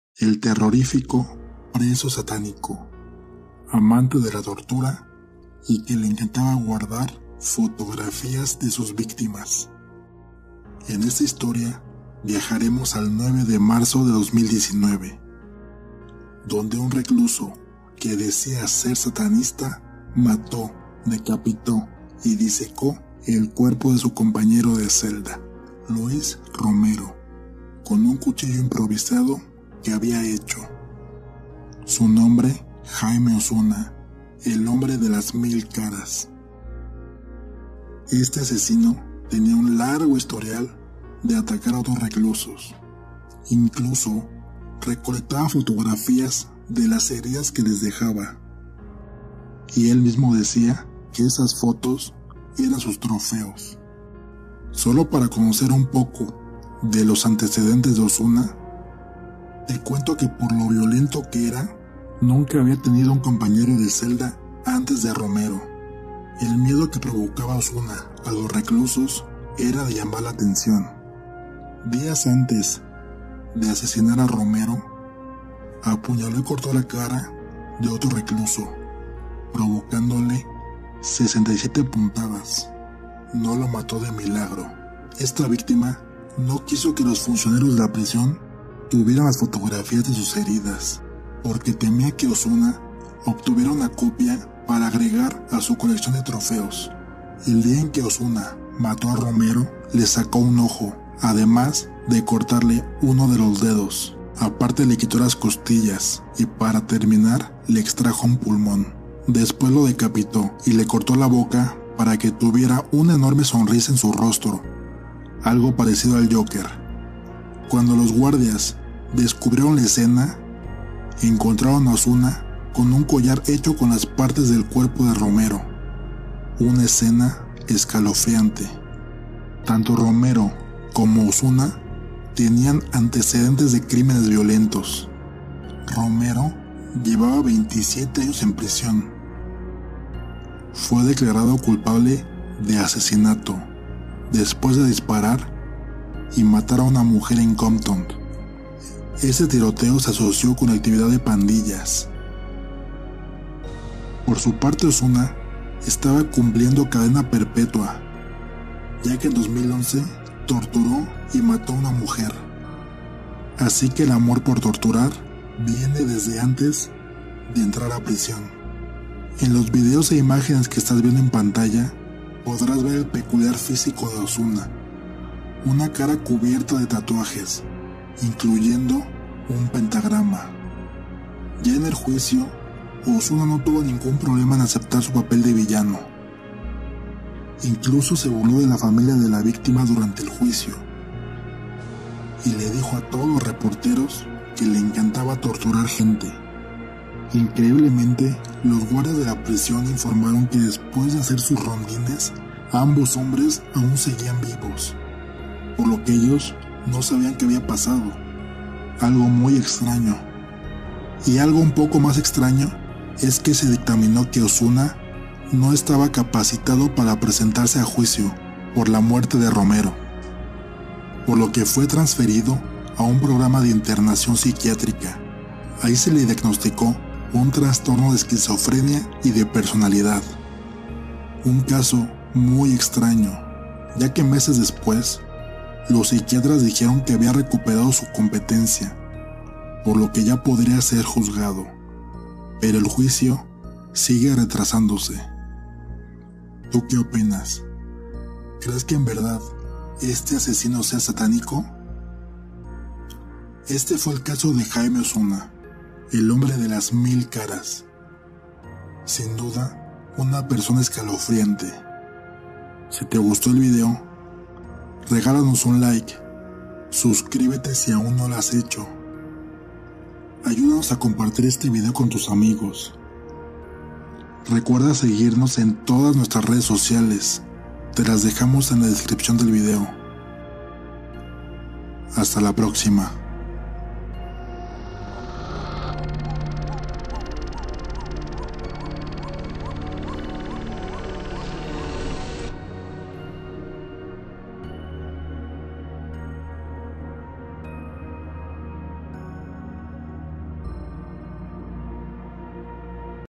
El asesino SATANISTA | Jaime Osuna | Hizo un COLLAR con las partes del CUERPO de su VÍCTIMA
información extra:

Bienvenido a Cápsulas de terror
Condenado a cadena perpetua por el asesinato de Yvette Peña, Jamie Osuna permanece a la espera de ser juzgado por decapitar a su compañero de celda en la prisión de Corcoran. "El hombre de las mil caras" tiene 34 años.
La mañana del 9 de marzo de 2019, los guardias de Corcoran revisaron su celda y descubrieron que Romero había sido decapitado, le habían sacado un ojo y le habían extraído otras partes del cuerpo. Osuna llevaba un collar hecho con partes del cuerpo de su compañero de celda.

“Jajaja” estaba escrito con sangre en todas las paredes. En una pared también estaba escrito “El hombre de las mil caras”.

Osuna consiguió la atención que siempre buscó. Su ficha policial se divulgó en todo el país y a través de artículos que detallaban su espantoso crimen se conocieron los antecedentes penales de Osuna.

¿DÓNDE ENCONTRARNOS?